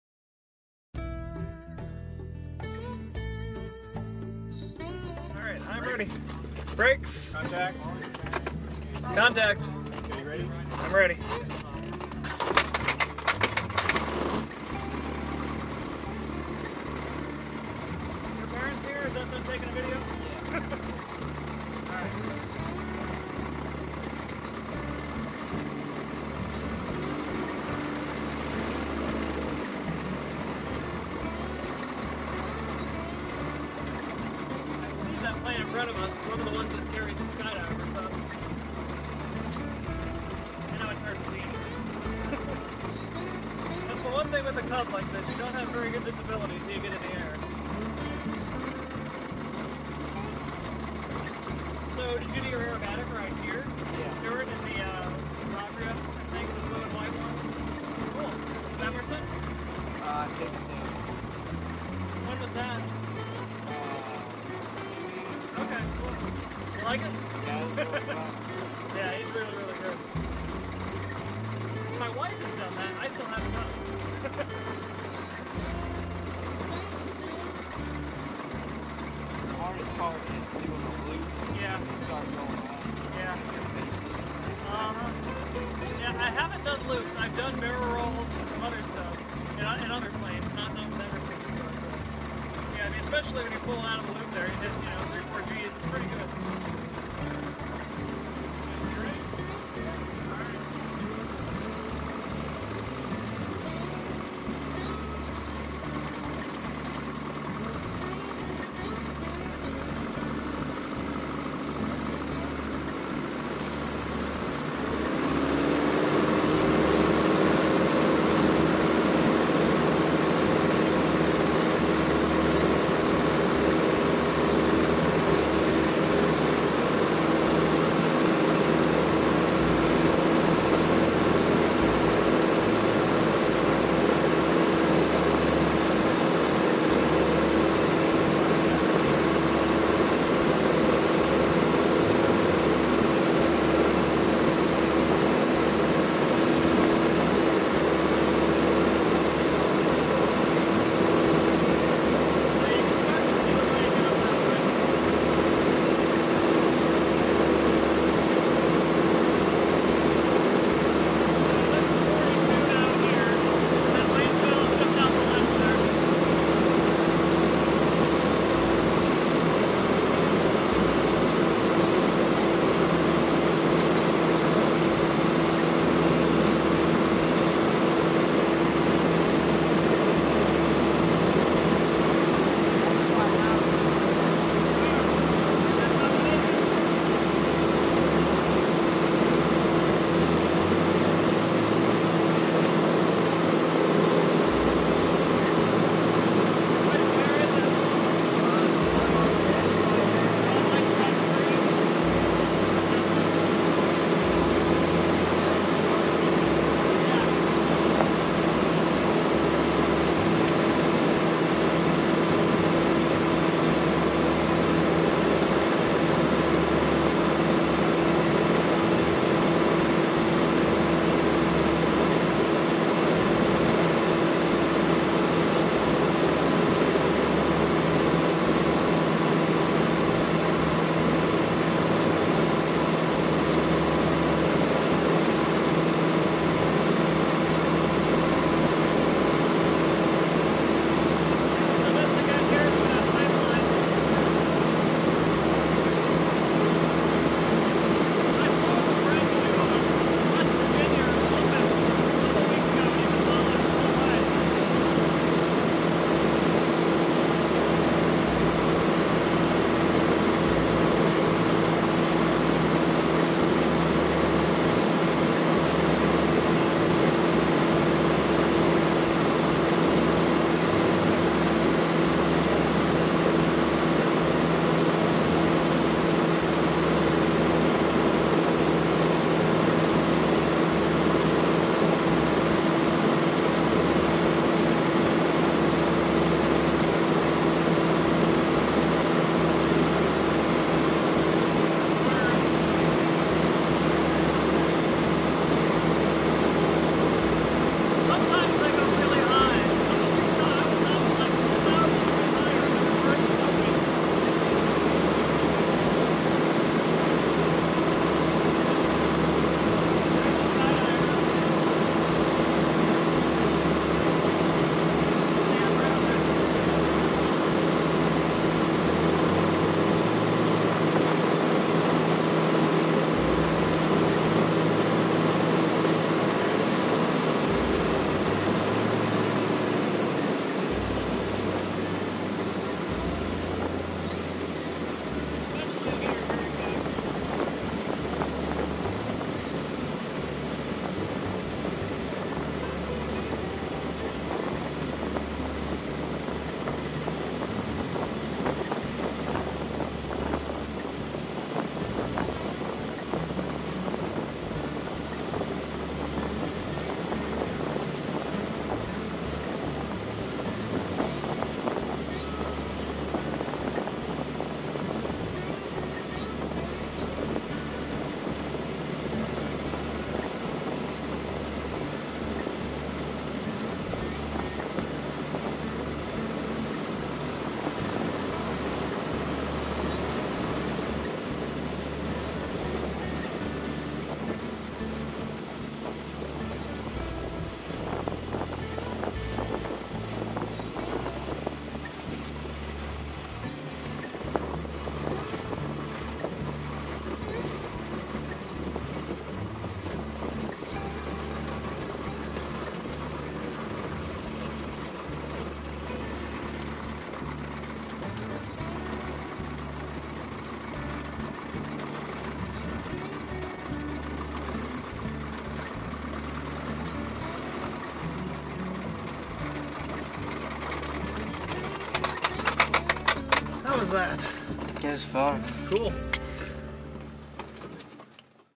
Chaston's First Cub Ride
I took Chaston flying during Red Stewart Airfield / EAA Chapter 284's annual pancake breakfast and Young Eagles rally over Labor Day Weekend 2013.